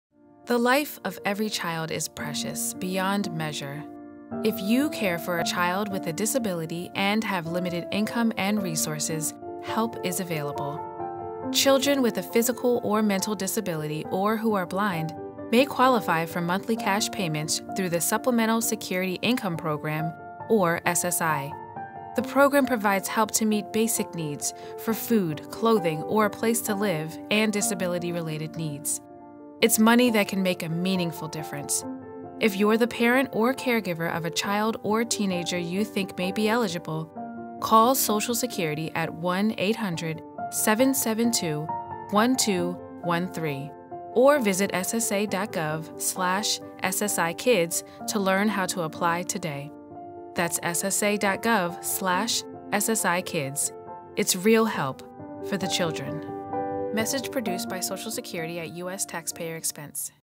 'The Life of a Child' - SSA PSA
‘The Life of a Child’
For low-income parents or caregivers of children and teenagers who have a disability, there is help in the form of Supplemental Security Income (SSI). The program provides money to help meet the child or teenager’s basic needs for things like food, clothing, a place to live, or disability-related needs.
Visit SSA.gov/SSIkids to learn if your child may qualify for SSI and how to apply.